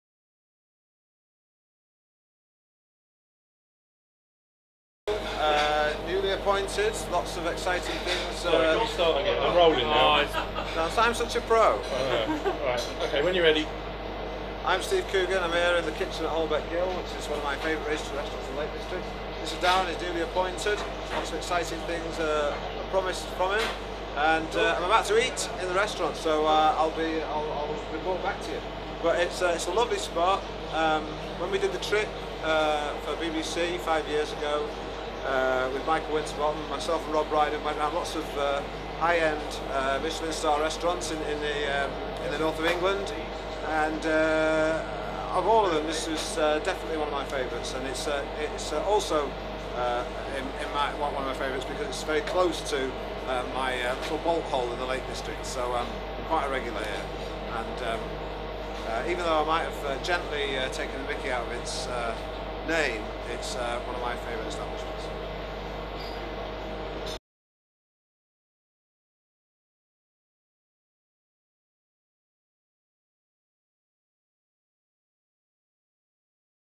Steve Coogan returns to Holbeck Ghyll
Comedy legend Steve Coogan returns to Holbeck Ghyll five years after the hotel featured in an episode of The Trip with Coogan and Rob Brydon. Steve meets new Head Chef Darren Comish in the hotel's noisy kitchen.